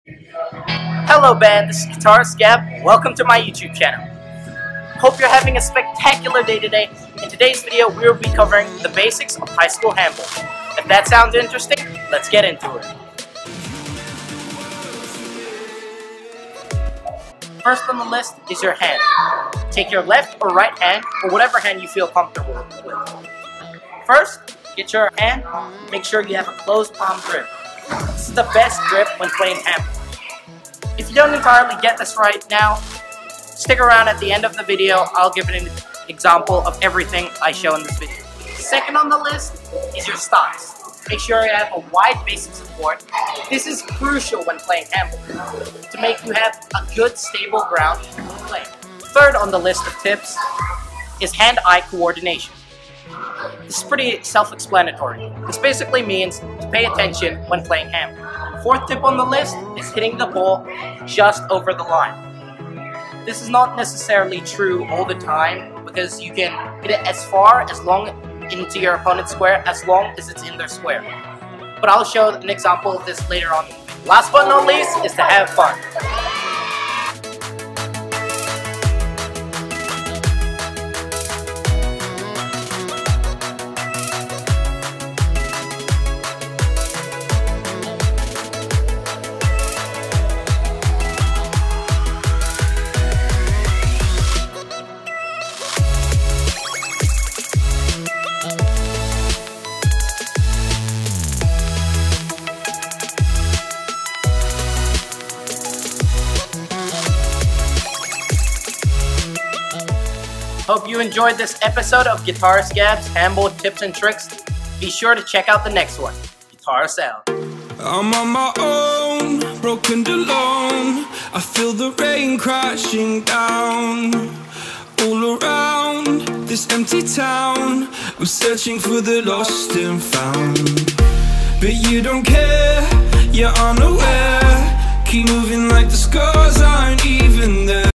Highschool Handball 101 | Guitarist Gab
Learn the basic of high school handball w/ Guitarist Gab.

Who is Guitarist Gab?

Hi,
I’m Guitarist Gab, I make handball tutorials. Not European handball or street handball but highschool handball. I give tips and tricks on handball from the basics of highschool handball to expert - level tactics.

Yours Truly,
Guitarist Gab